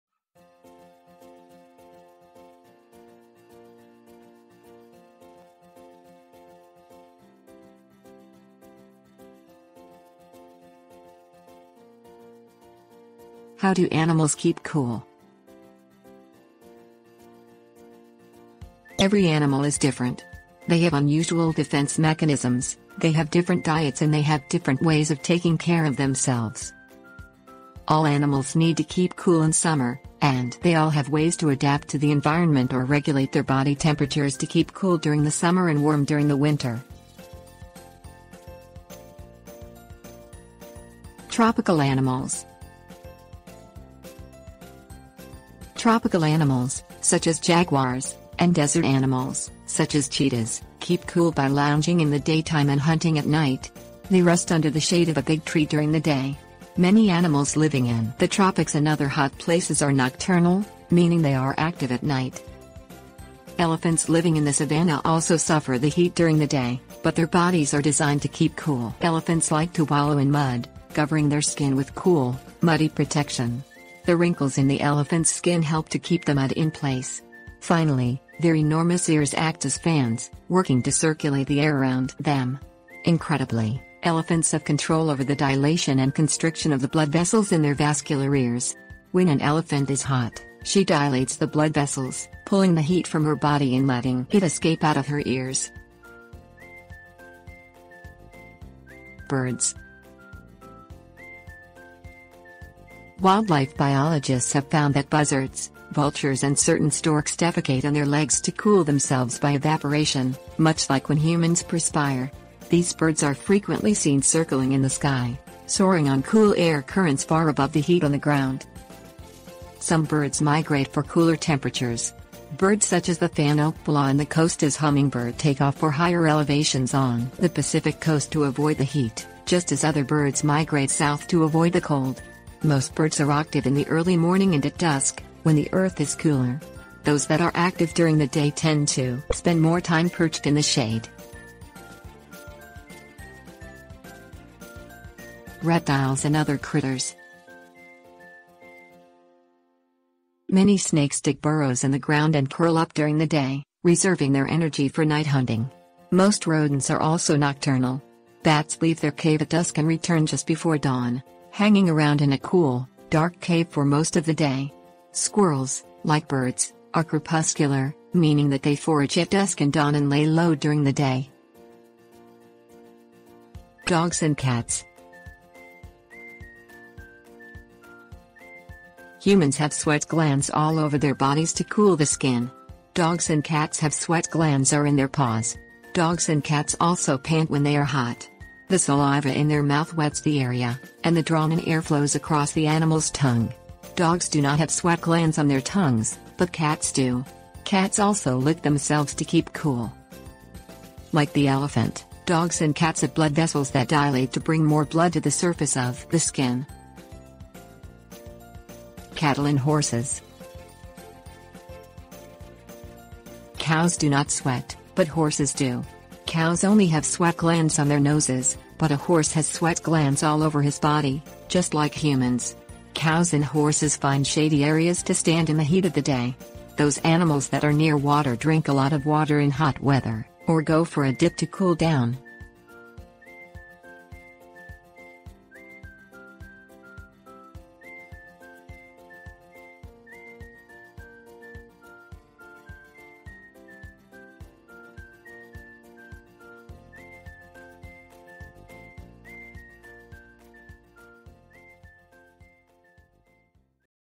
How Do Animals Keep Cool?
How Do Animals Keep Cool?. Every animal is different. They have unusual defense mechanisms, they have different diets and they have different ways of taking care of themselves.All animals need to keep cool in summer, and they all have ways to adapt to the environment or regulate their body temperatures to keep cool during the summer and warm during...

Table of contents How Do Animals Keep Cool?
Tropical Animals 00:39
Birds 01:38
Reptiles and other Critters 02:24
Dogs and Cats 02:53
Cattle and Horses 03:28